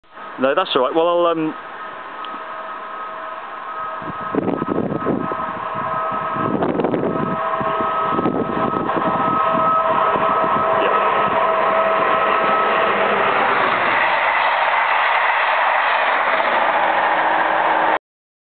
67018 & 67016 near Brent Knoll.AVI
Video of 67018 and 67016 passing Wick near Brent Knoll with the 2U24 16.16 Taunton - Cardiff train on 2nd August 2010.